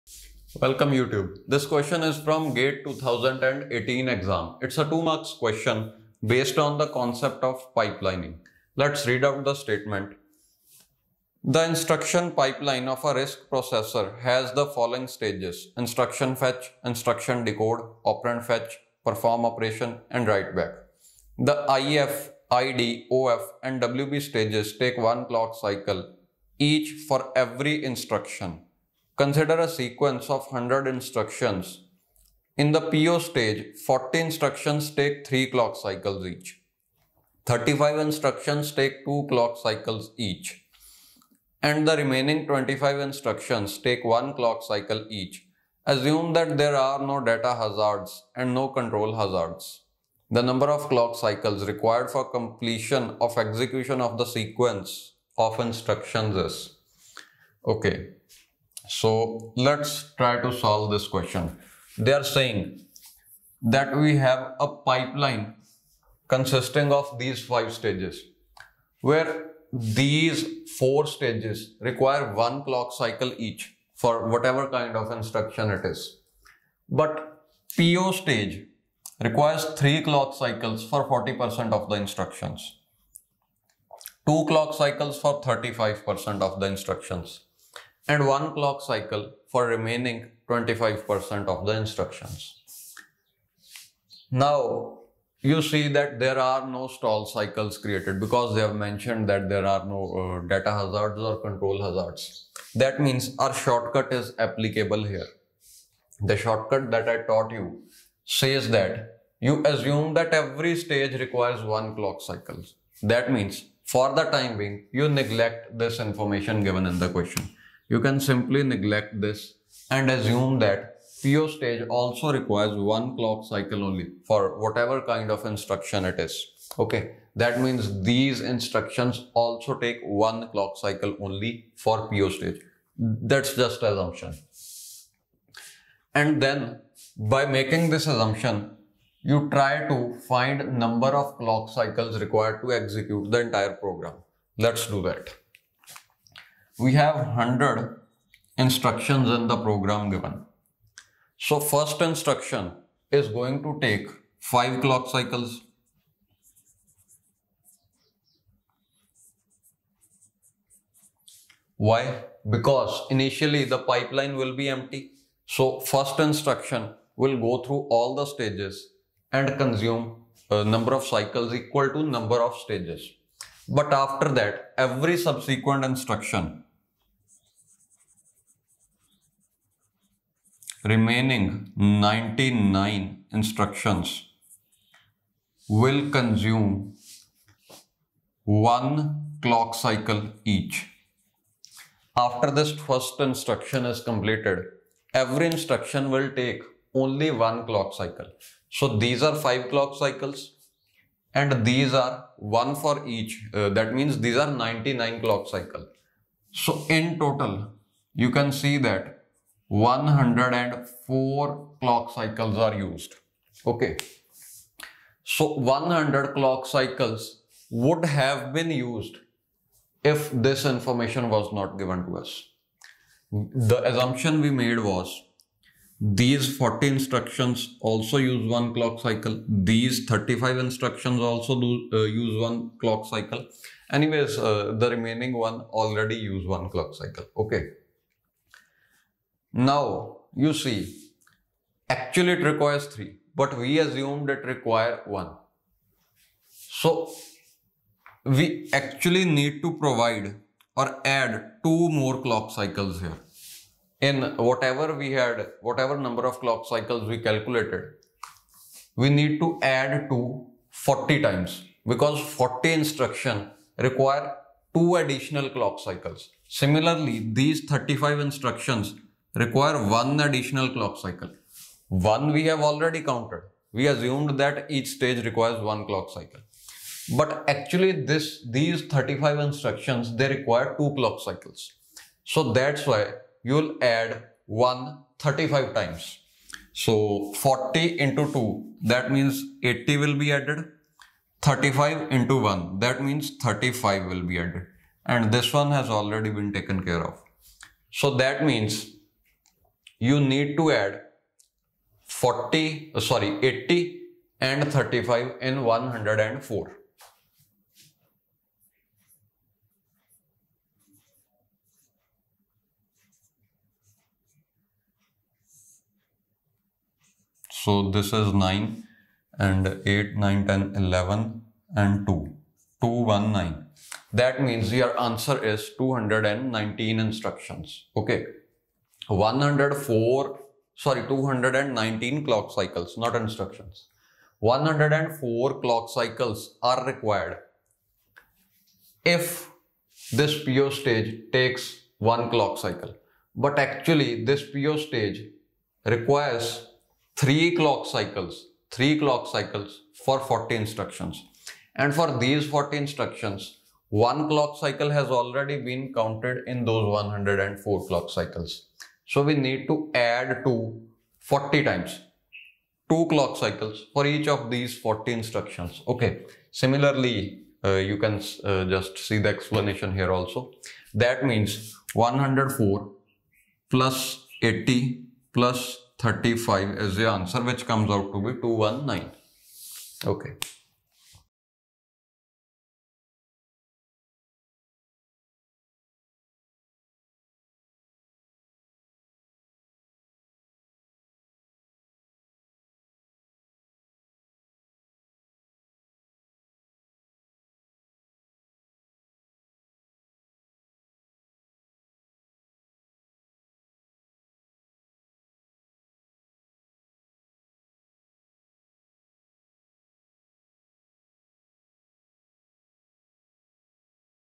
Gate 2018 pyq CAO | The instruction pipeline of a RISC processor has the following stages:
The instruction pipeline of a RISC processor has the following stages: Instruction Fetch (IF), Instruction Decode (ID), Operand Fetch (OF), Perform Operation (PO) and Writeback (WB), The IF, ID, OF and WB stages take 1 clock cycle each for every instruction. Consider a sequence of 100 instructions. In the PO stage, 40 instructions take 3 clock cycles each, 35 instructions take 2 clock cycles each, and the remaining 25 instructions take 1 clock cycle each. Assume that there are no data hazards and no control hazards.

The number of clock cycles required for completion of execution of the sequence of instruction is ______ .